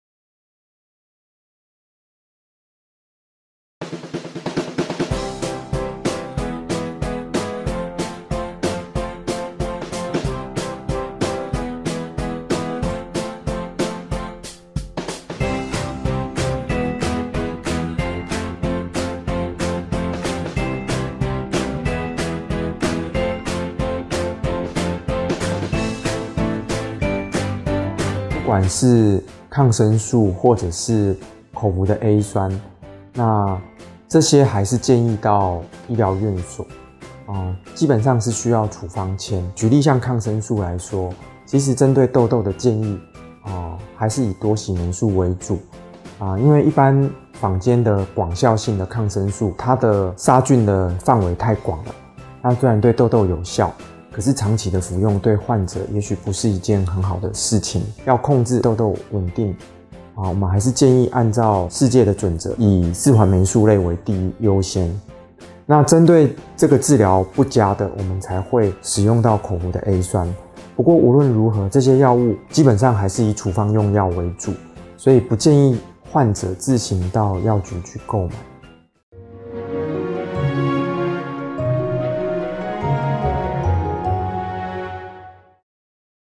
痘痘｜治療痘痘自行用藥 恐造成身體不適？！ft.皮膚科何文藻醫師【ME美醫誌】
坊間的診所都會販賣迅速見效的痘痘藥品來治療痘痘，但是這些宣稱療效的產品是可以用嗎？皮膚科何文藻醫師表示不管是抗生素或口服A酸，都還是要到醫療院所看診後拿到處方籤用藥，因為個人並不熟悉成分和使用的次數和時間，不建議自行到藥房亂購買。

各節重點：
00:28 痘痘藥適合藥局自取嗎？(抗生素、酸類藥品)
00:58 控制痘痘治療方法

延伸閱讀：
痘痘｜膿皰結節清潔不易 藥物才是改善手段 ft.皮膚科何文藻醫師【ME美醫誌】